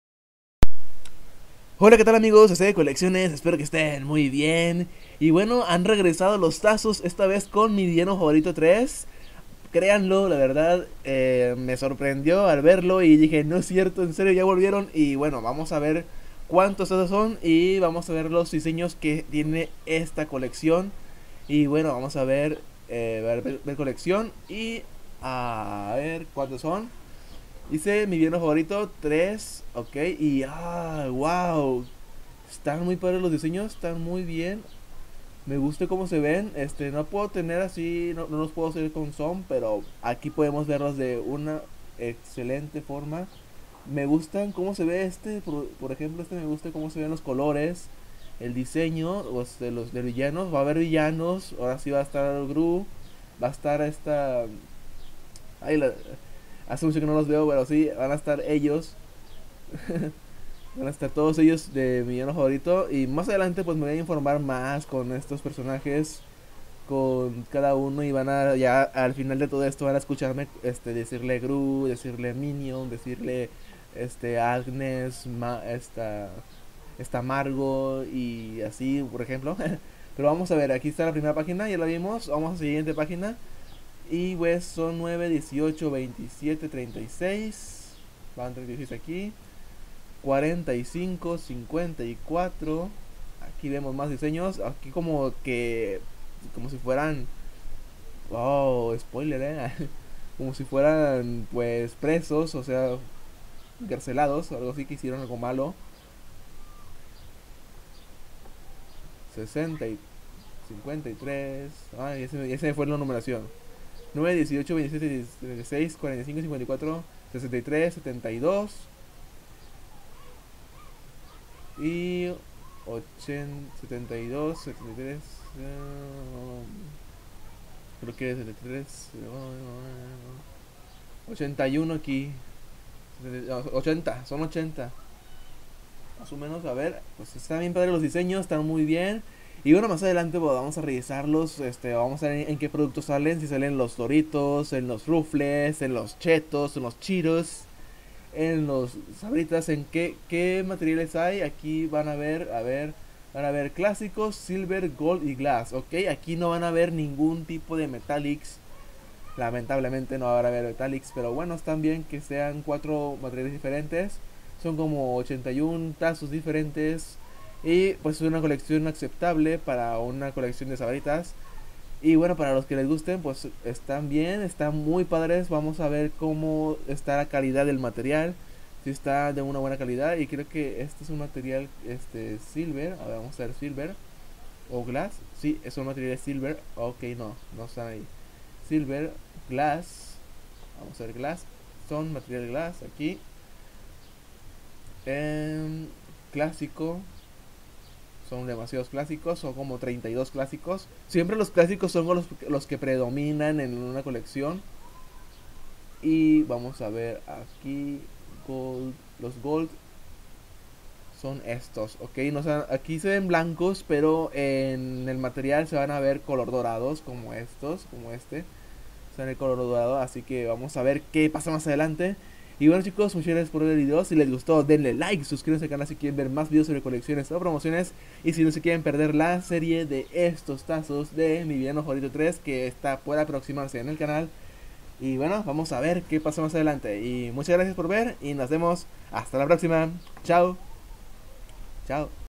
Tazos de mi villano favorito 3 | C-de Colecciones
Vuelven los tazos de mi villano favorito 3 con 80 tazos diferentes en 4 materiales!

Más?